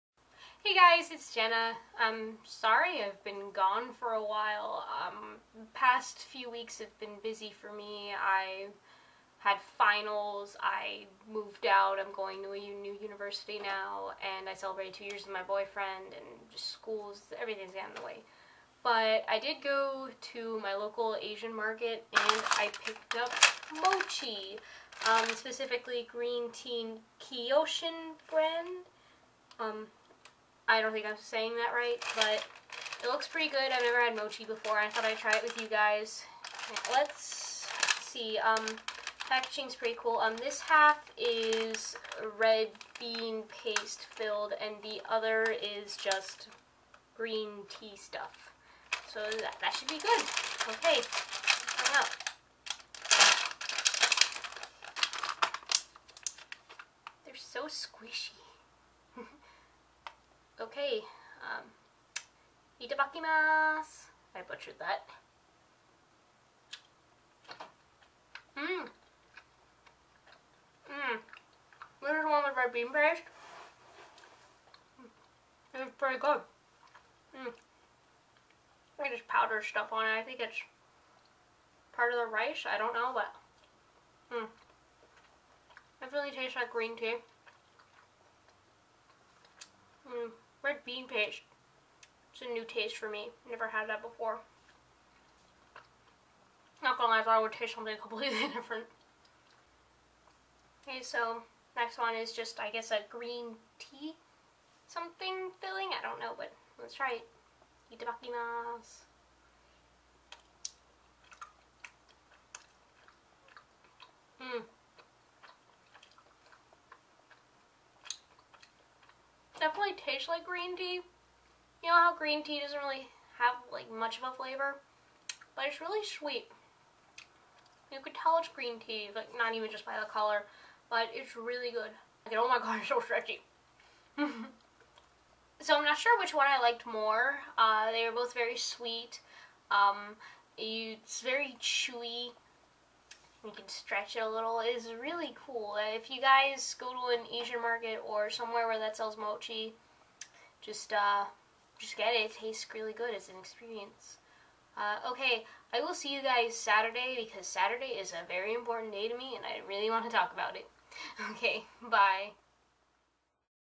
Trying Mochi For The First Time
I'm back! And to be more specific it's green tea daifuku.